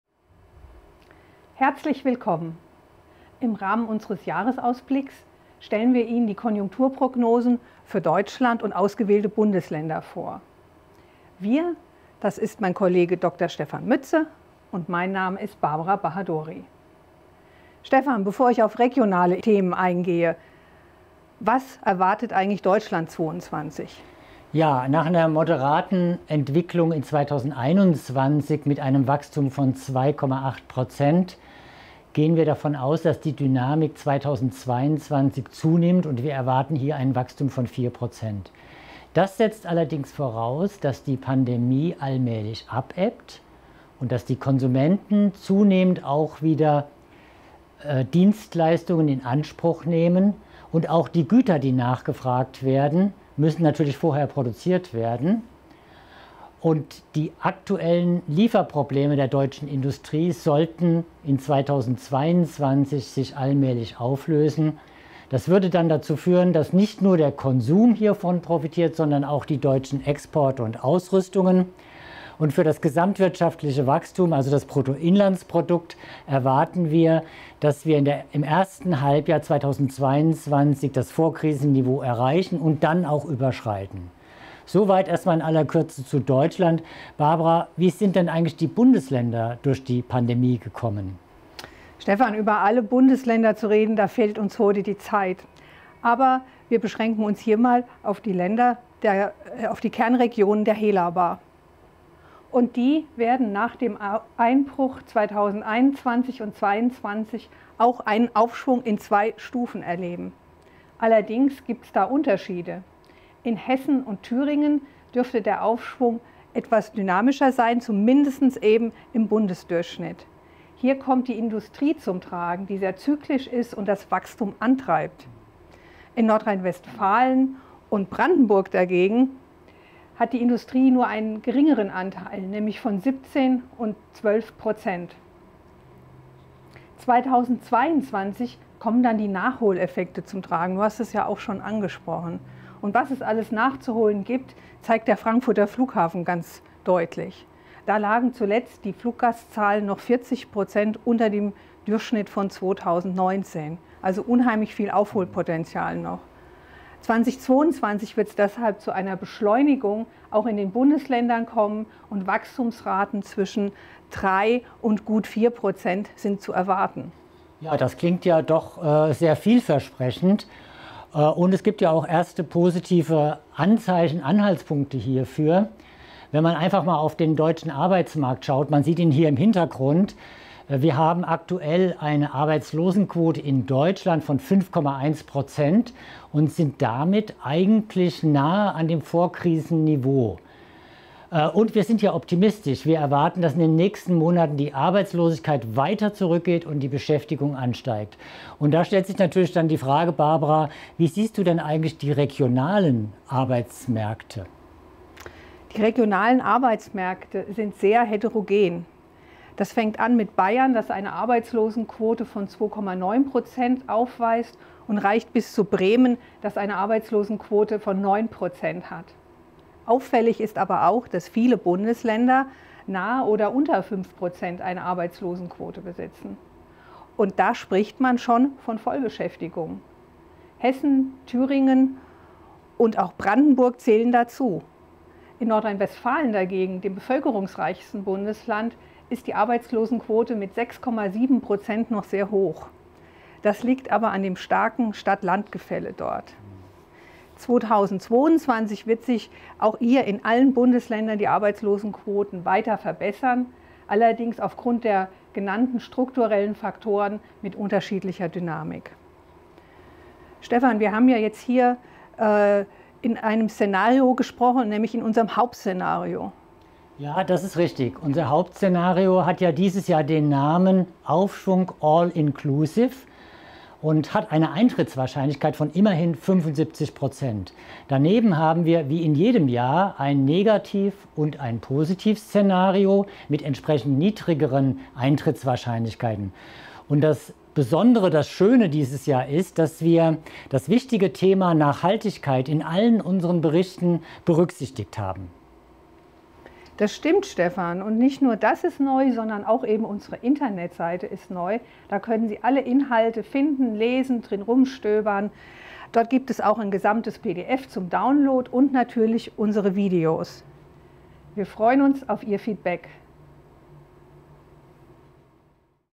Märkte und Trends 2022: Deutschland und ausgewählte Bundesländer
Die deutsche Konjunktur erhält zusätzliche Impulsgeber. Welche Wachstumsraten können die Kernregionen der Helaba erwarten? Wie groß sind die Unterschiede zwischen den regionalen Arbeitsmärkten?